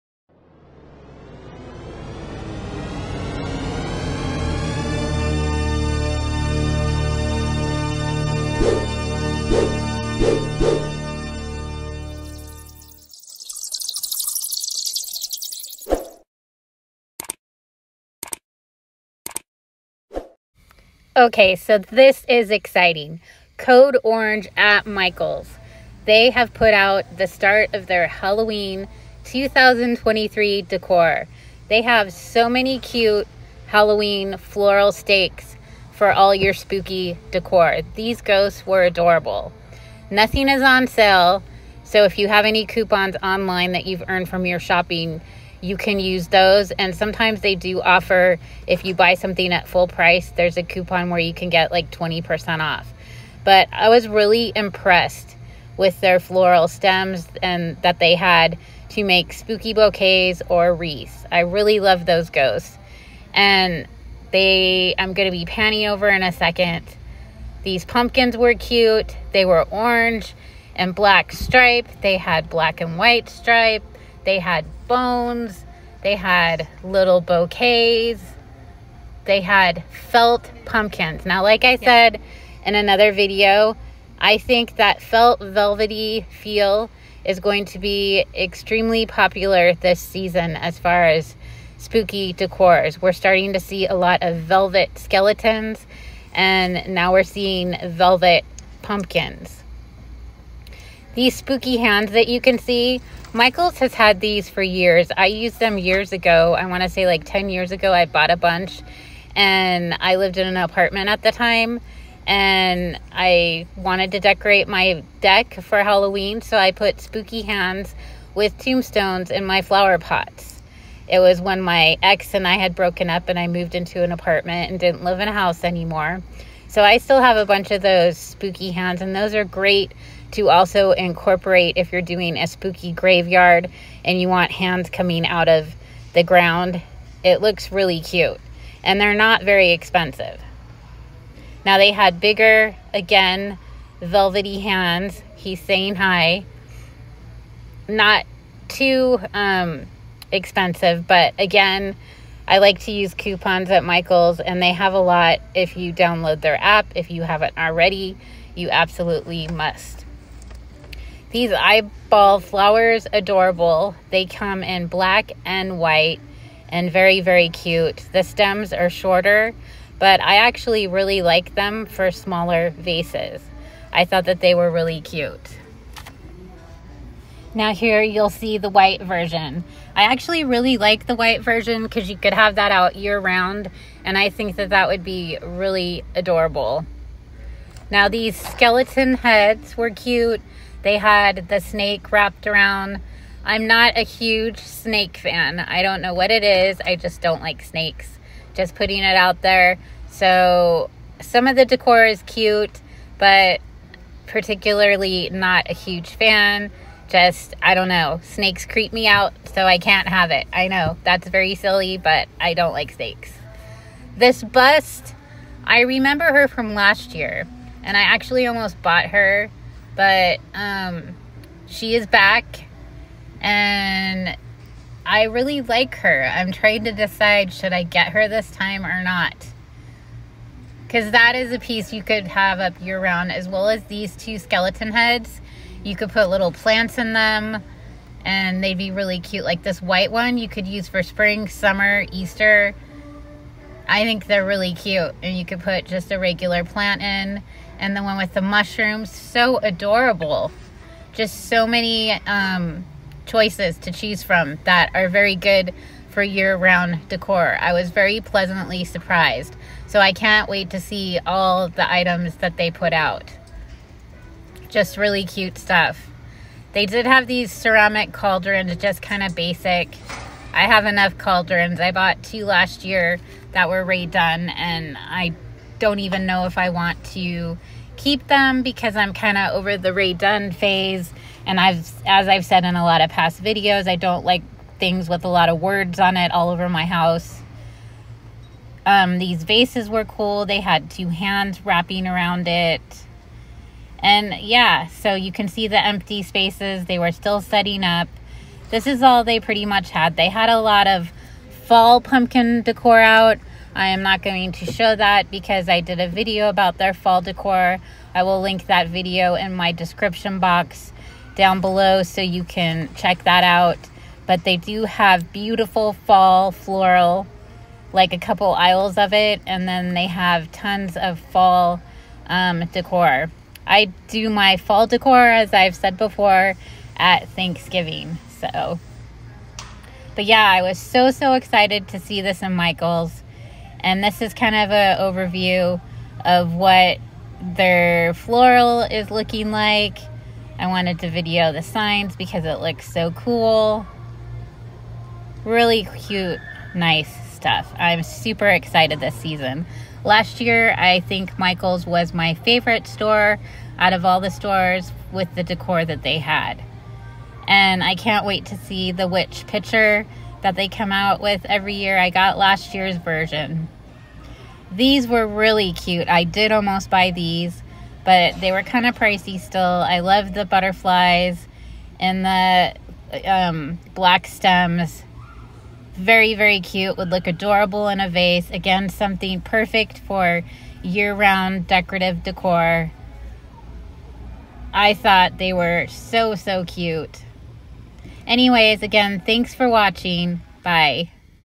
New Michaels Halloween 2023 it’s here!!
I decided to stop in to Michaels today to check to see if they had any Halloween decor.  YES they did. So beyond excited 🎃🎃🎃